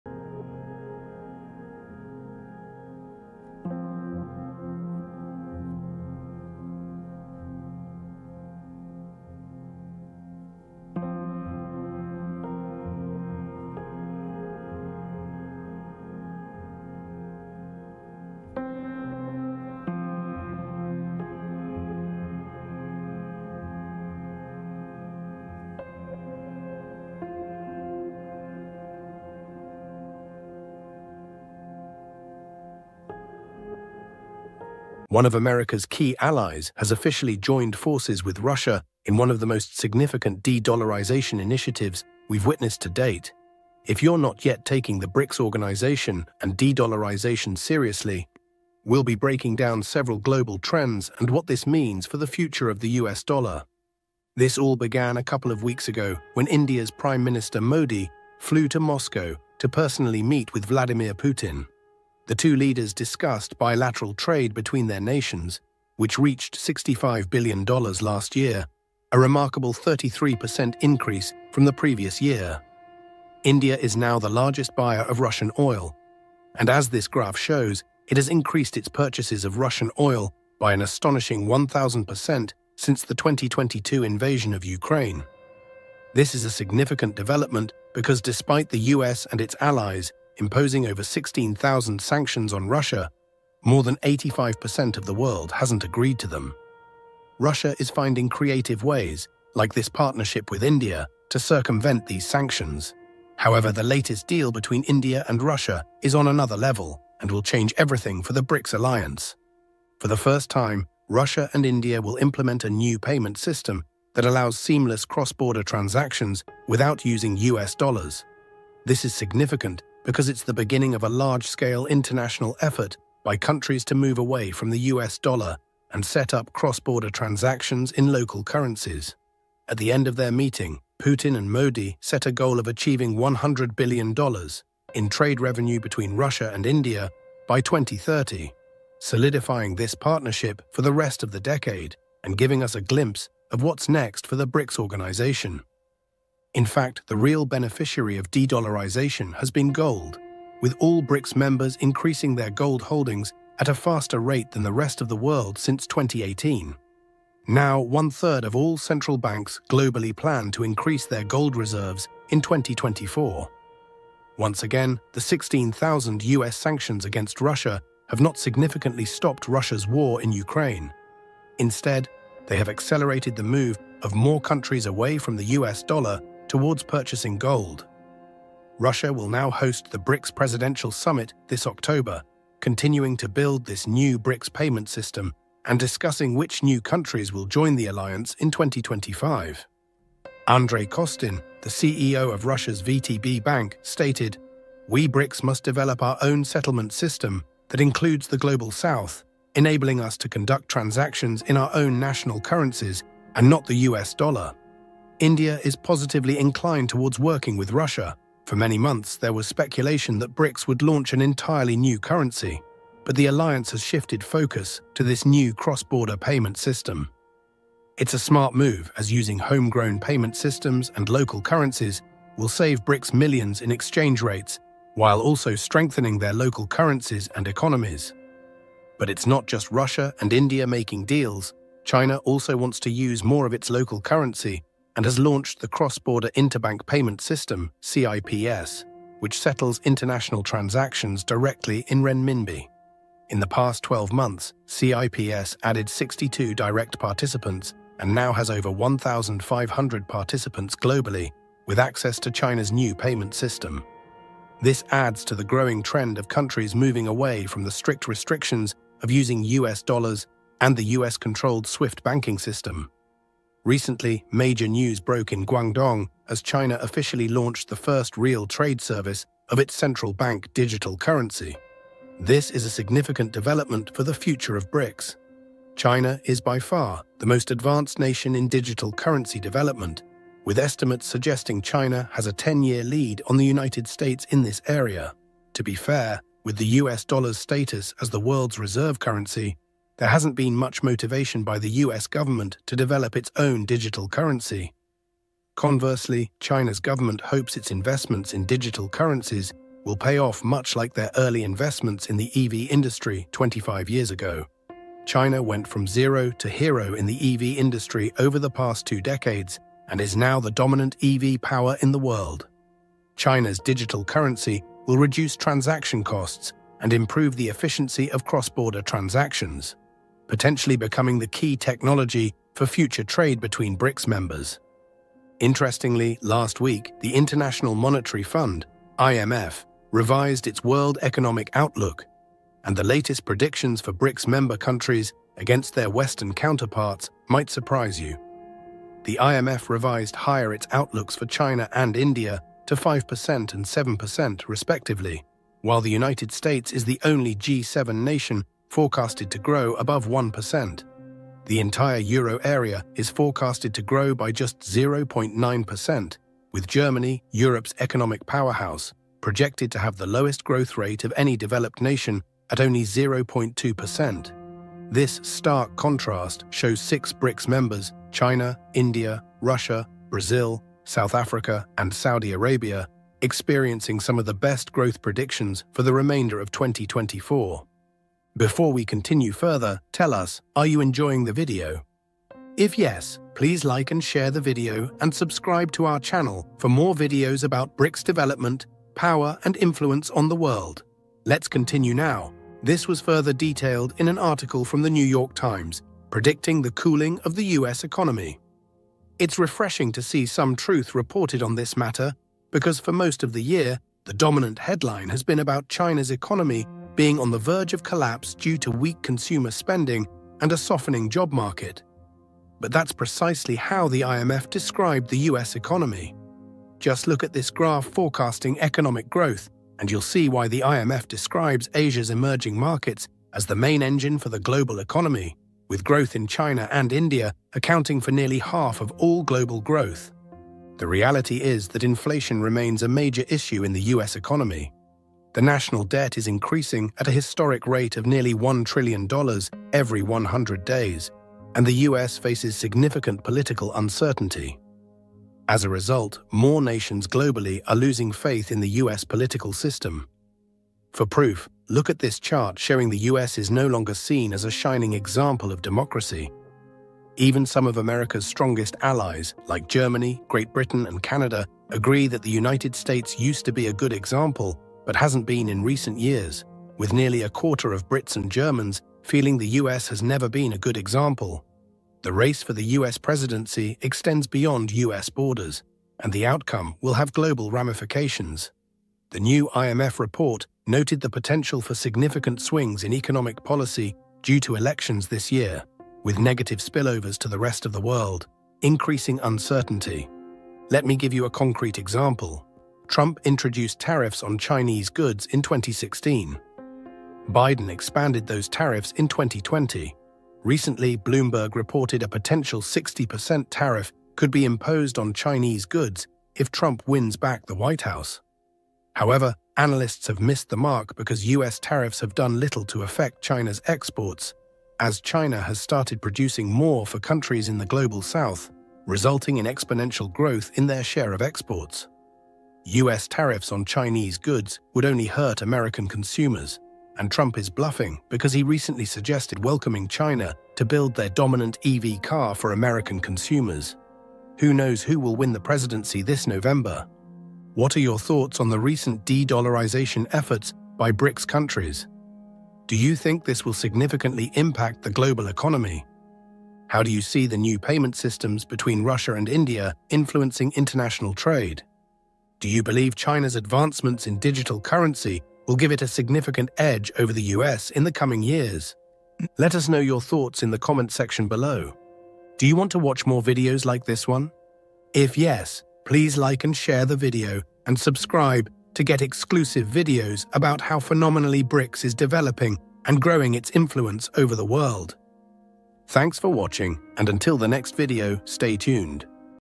BRICS New Deal Just Shocked the World! Dollar's End Finally!
In this video, we delve into the groundbreaking developments surrounding the BRICS nations and their bold move to challenge the dominance of the US Dollar. Russia and India have just launched a new payment system that allows both countries to avoid using the US Dollar in foreign trade. Adding to the seismic shift, China is not only joining this program but has also launched its own payment system and a digital currency project. This collective effort marks a significant move by these powerful nations to reduce their reliance on the US Dollar, a process known as de-dollarization. As more countries around the world follow suit, the implications for the global economy are profound. Join us as we explore the details of this BRICS New Deal, the ongoing de-dollarization trend, and their potential to reshape the financial landscape.